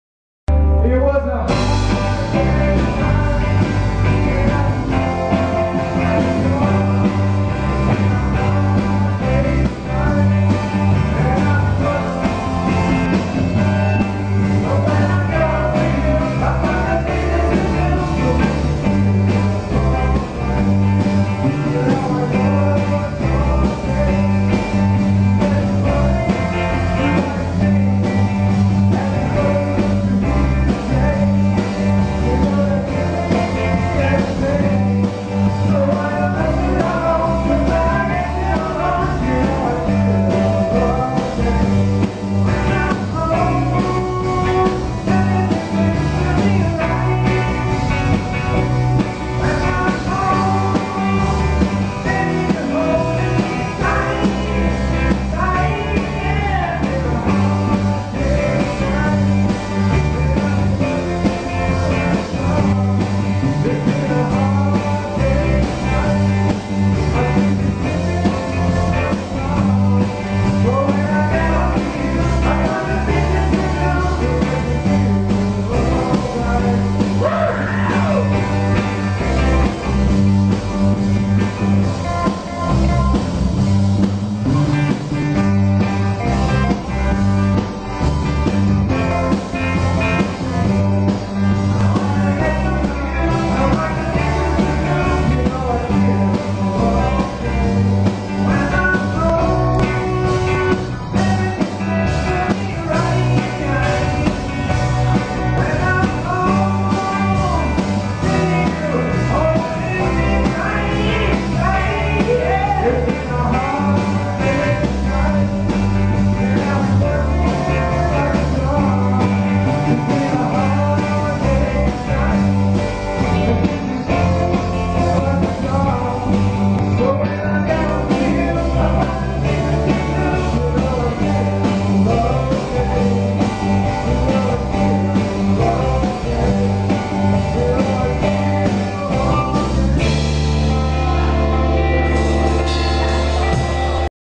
Banda Night Before
Ensaio final para apresentação da Banda Night Before